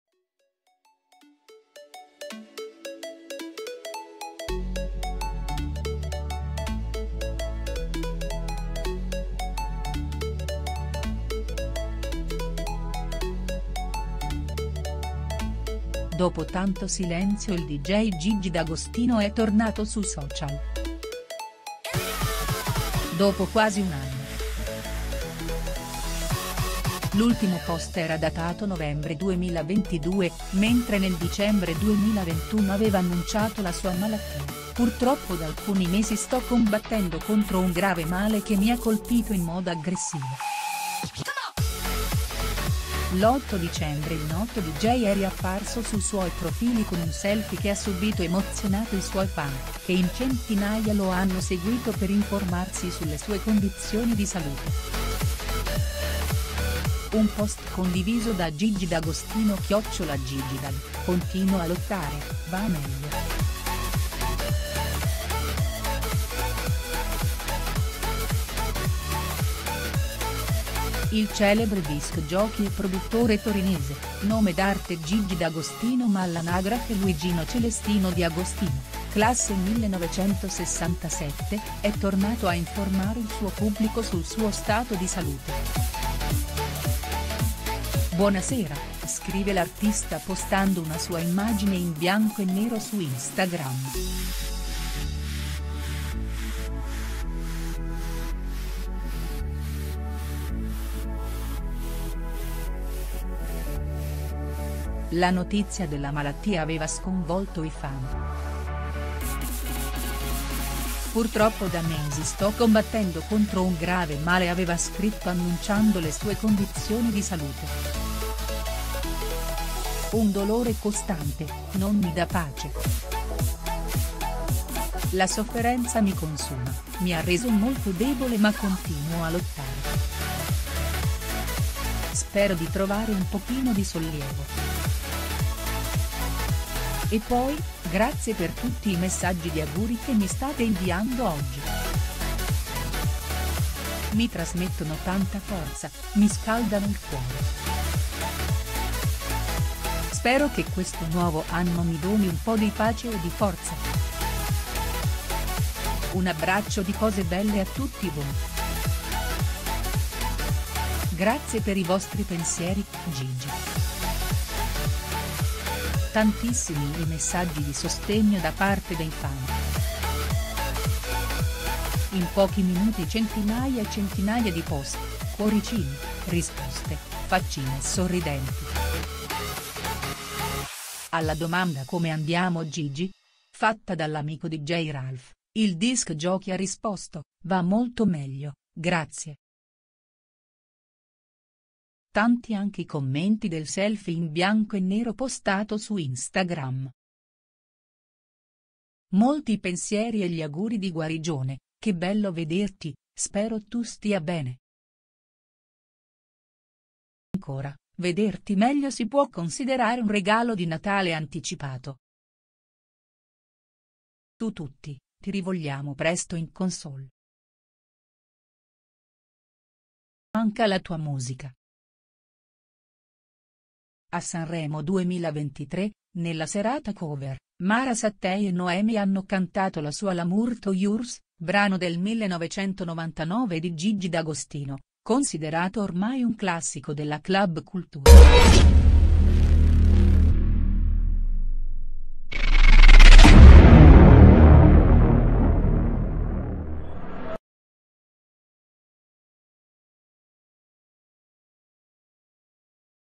Gigi D’Agostino rompe il silenzio sui social: “Buonasera”. E i fan gli rispondono con affetto
Gigi D’Agostino rompe il silenzio sui social: “Buonasera”. E i fan gli rispondono con affetto Dopo tanto silenzio il dj Gigi D’Agostino è tornato sui social. Dopo quasi un anno. L'ultimo post era datato novembre 2022, mentre nel dicembre 2021 aveva annunciato la sua malattia: «Purtroppo da alcuni mesi sto combattendo contro un grave male che mi ha colpito in modo aggressivo». L’8 dicembre il noto dj è riapparso sui suoi profili con un selfie che ha subito emozionato i suoi fan, che in centinaia lo hanno seguito per informarsi sulle sue condizioni di salute. Un post condiviso da Gigi D'Agostino (@gigidag) "Continuo a lottare, va meglio”. Il celebre disc jockey e produttore torinese, nome d’arte Gigi D’Agostino ma all’anagrafe Luigino Celestino Di Agostino, classe 1967, è tornato a informare il suo pubblico sul suo stato di salute. “Buonasera”, scrive l'artista postando una sua immagine in bianco e nero su Instagram. La notizia della malattia aveva sconvolto i fan. "Purtroppo da me